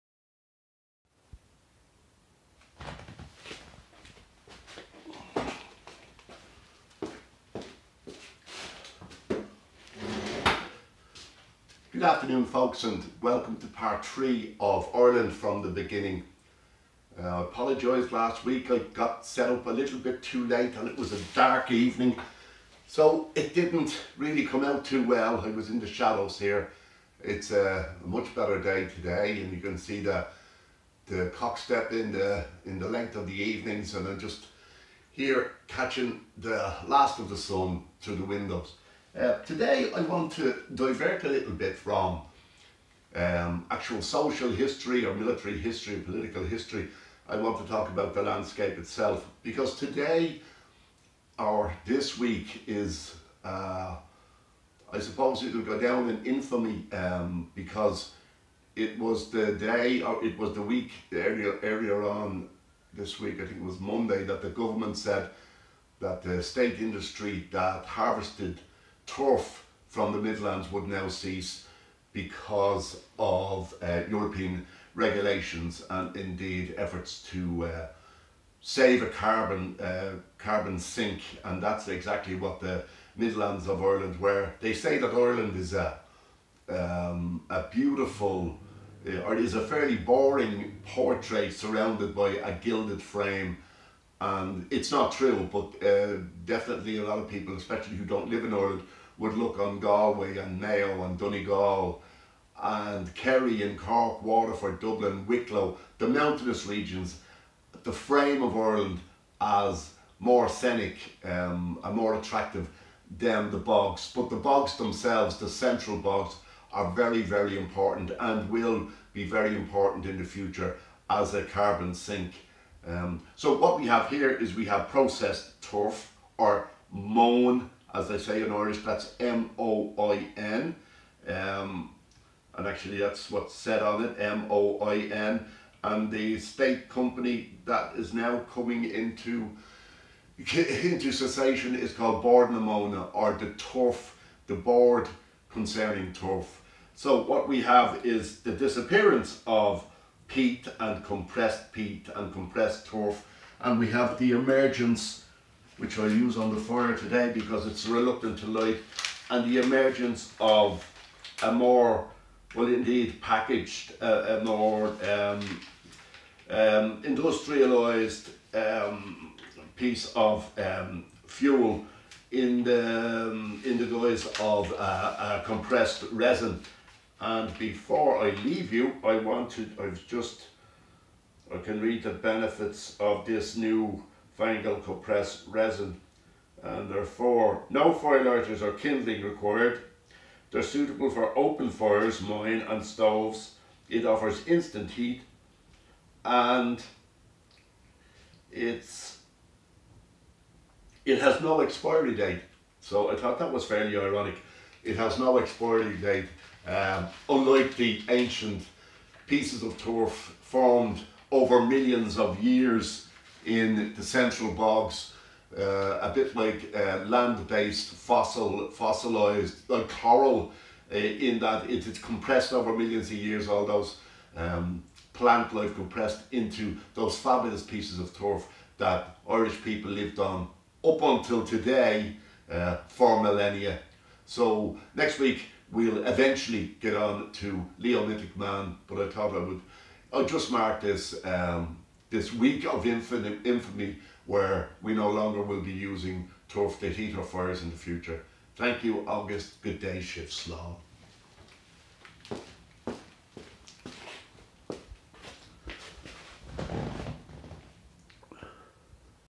Ireland From The Beginning Part Three
The cessation of turf harvesting by the Irish State